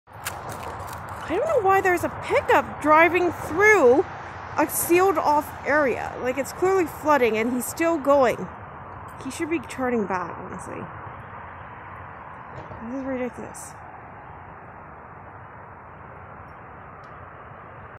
Amphibious Pick Up Truck?!
So we were walking to the dog park went we spotted a pick up truck attempting to drive through the closed off and totally flooded road. The pick up truck driver eventually noticed how deep the water was getting and turned around. This section of Gilkison Street has been closed for like 2 weeks now. Not even 10 minutes after arriving at the dog park and showing a dozen people this video clip, 4 fire trucks, 1 police vehicle and 1 ambulance came rushing through. Apparently someone had noticed the pick up truck and called the emergency services. The police officer saw this video twice and just shook his head. The stupidity of some people!!!